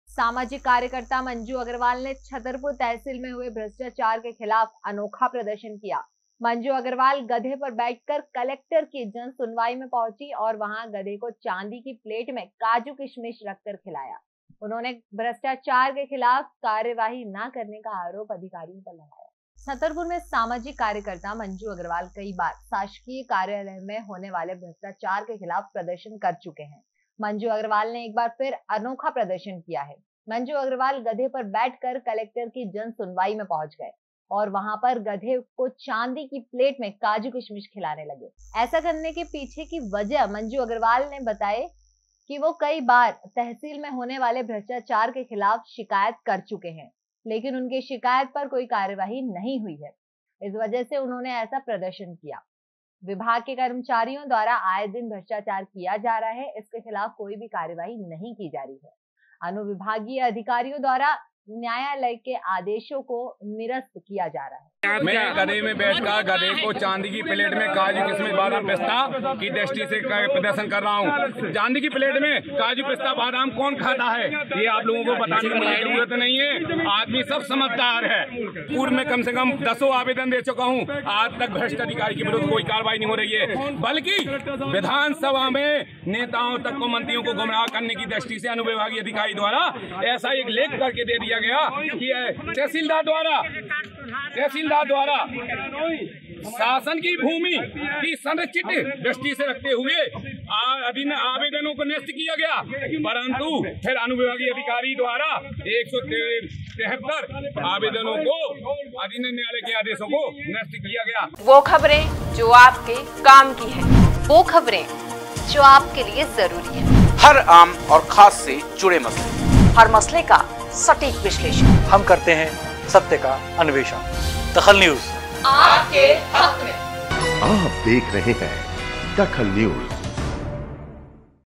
DAKHAL NEWS सामाजिक कार्यकर्ता ने किया गधे पर बैठकर प्रदर्शन
सामाजिक कार्यकर्ता ने किया गधे पर बैठकर प्रदर्शन
तहसीलदार पर भ्रष्टाचार करने का आरोप लगाया